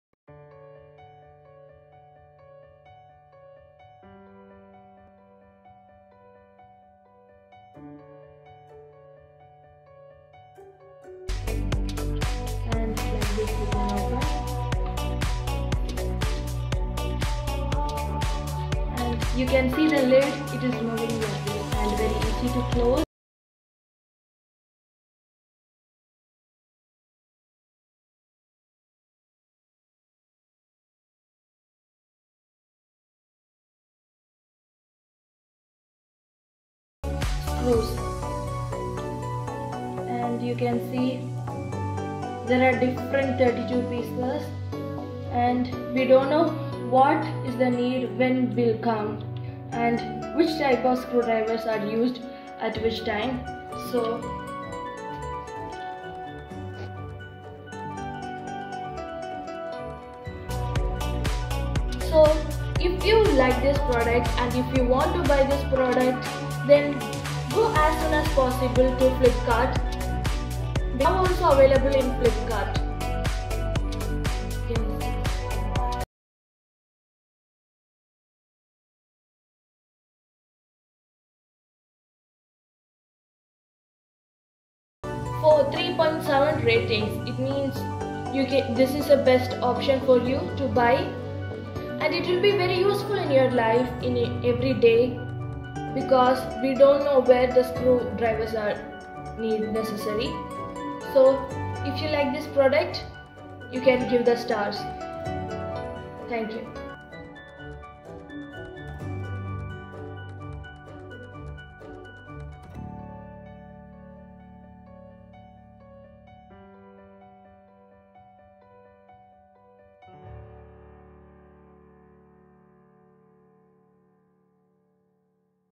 How it works & review of Jackly CT 6032 32 In 1 Interchangeable Precise Screwdriver Tool Set
How the Jackly CT 6032 32 In 1 Interchangeable Precise Screwdriver Tool Set works. Complete review of screwdriver set. Review submitted by Rajesh on Revexpo.com